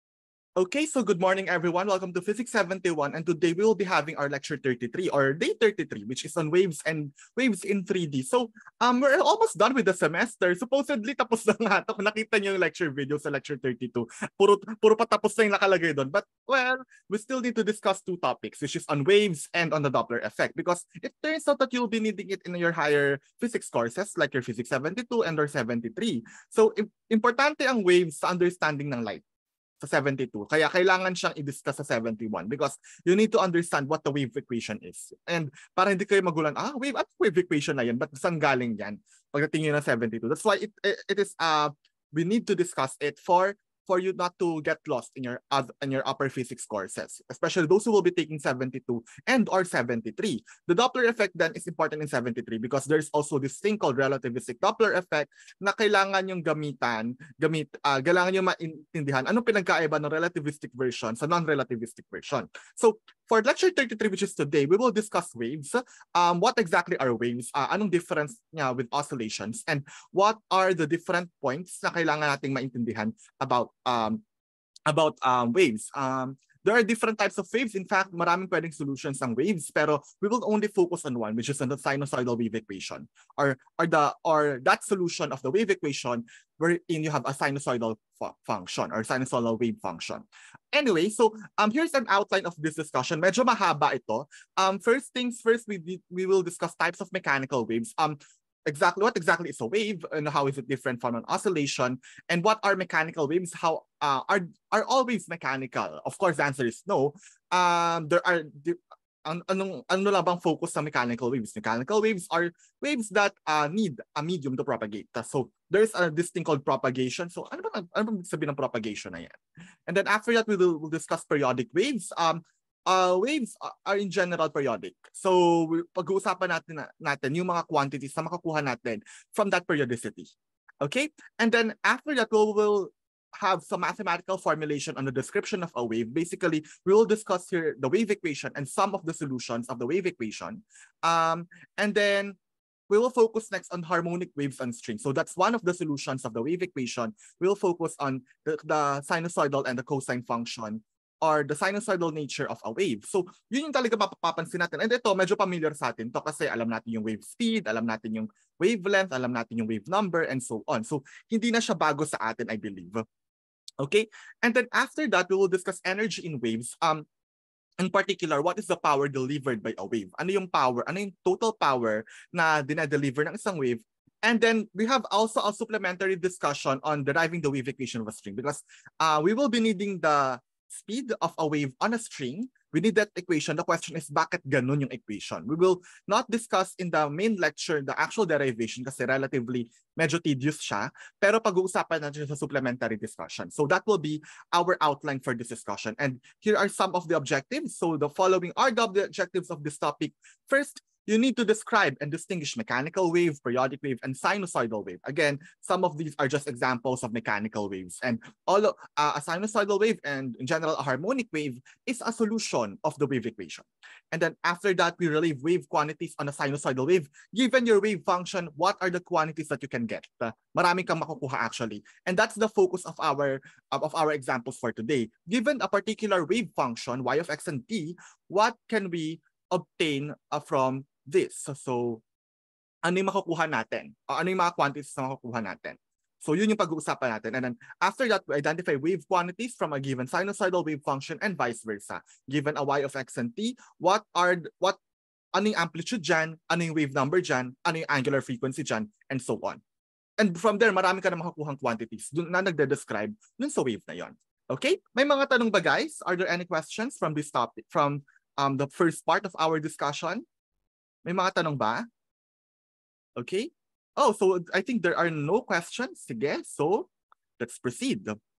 Physics 71 Day 33 [Part 1/10] | 'Di pa pala tayo tapos? Kala ko tapos na?
In this video, we discuss Day 33 which is on Waves and Waves on 3D. The topic is important since the wave equation will be relevant in the study of light.